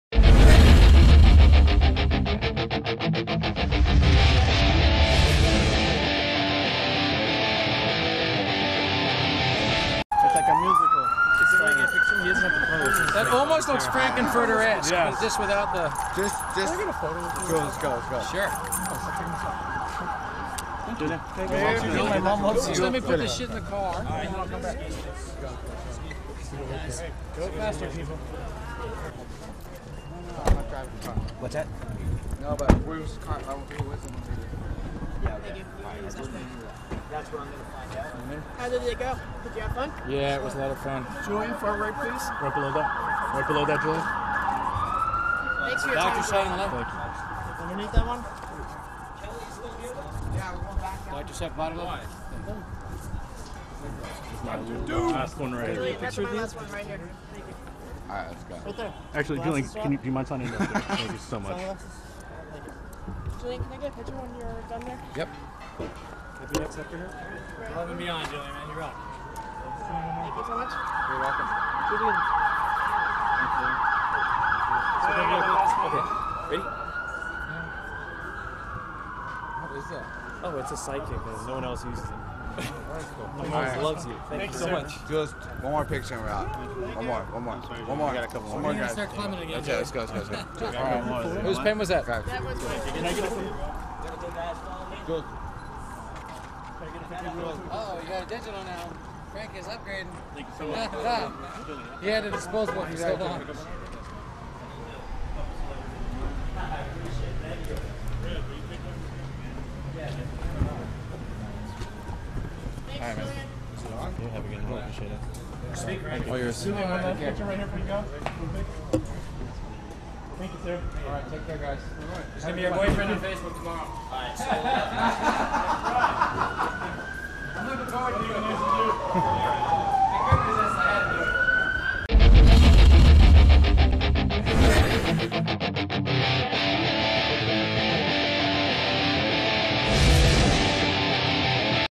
Julian McMahon Signing Autographs at Rocky Horror Picture Show
Julian signing autographs at Wiltern Theater after Rocky Horror Picture Show performance.  Such a sweetheart!!